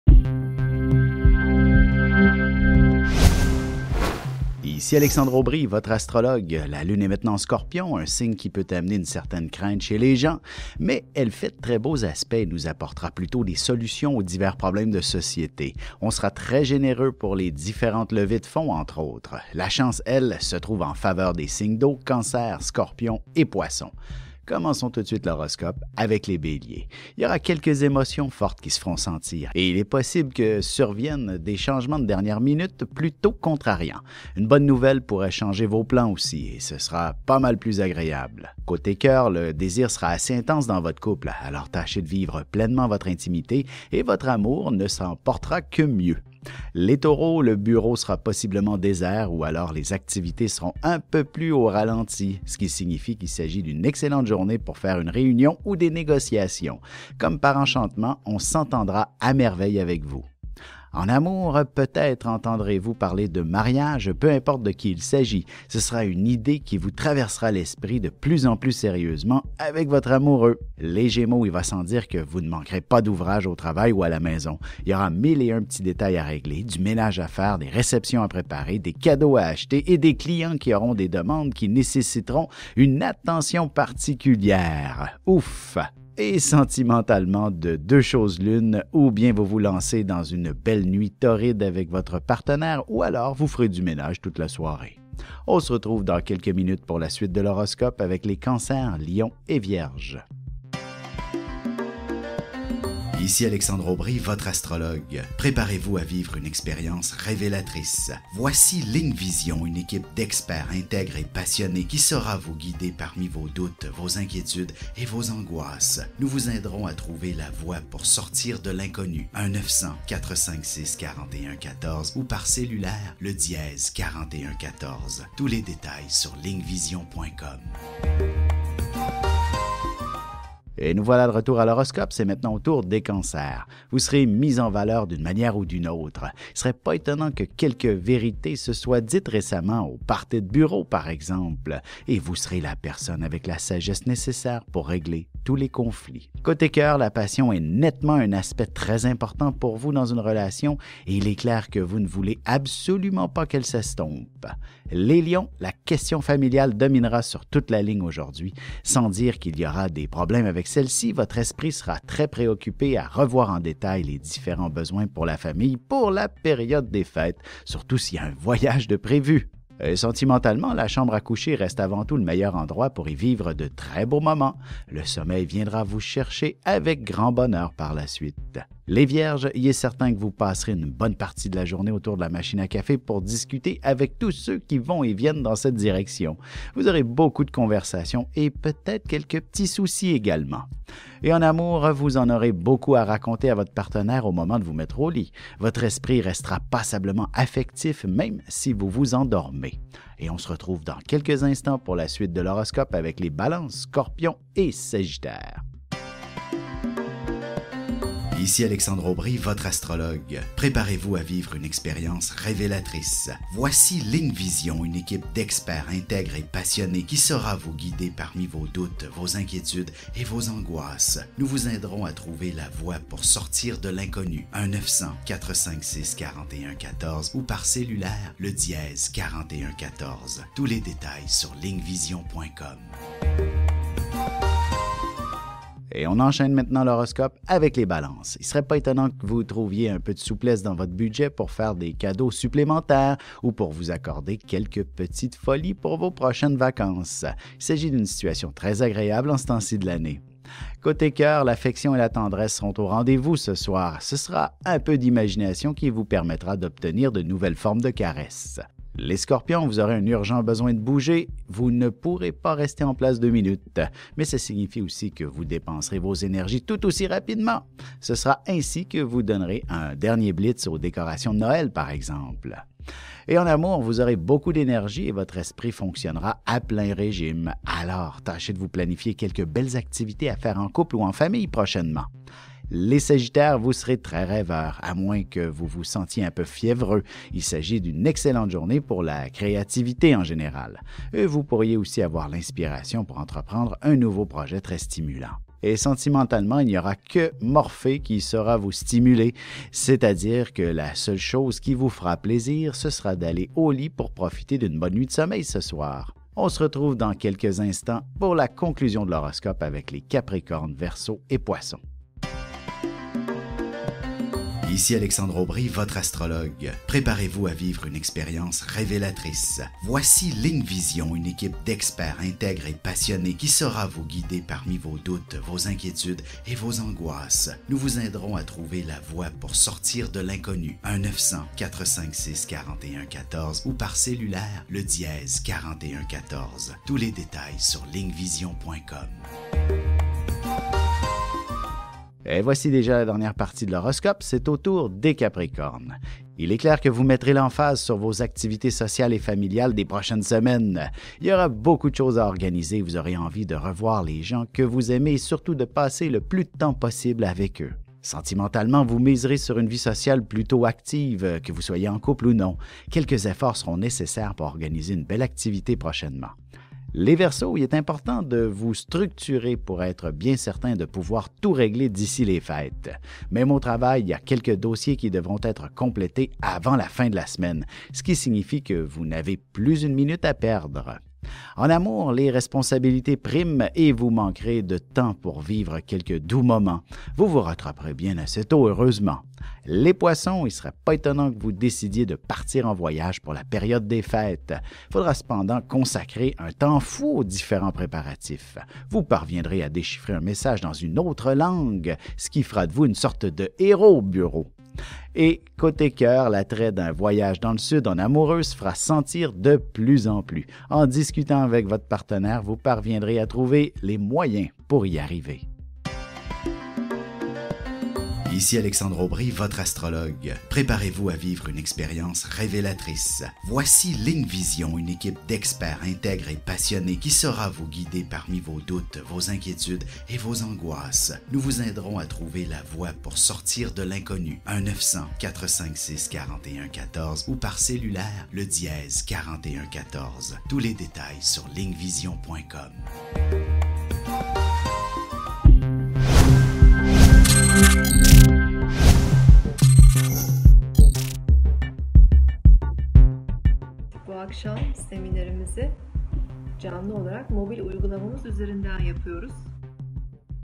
LUNDI 7 DÉCEMBRE 2015
Horoscope du jour avec Alexandre Aurby, LUNDI 7 DÉCEMBRE 2015

La lune est maintenant en Scorpion, un signe qui peut amener une certaine crainte chez les gens. Mais elle fait de très beaux aspects et nous apportera plutôt des solutions aux divers problèmes de société. On sera très généreux pour les différentes levées de fond et autres. La chance se trouve en faveur des signes d’eau (Cancer, Scorpion et Poissons).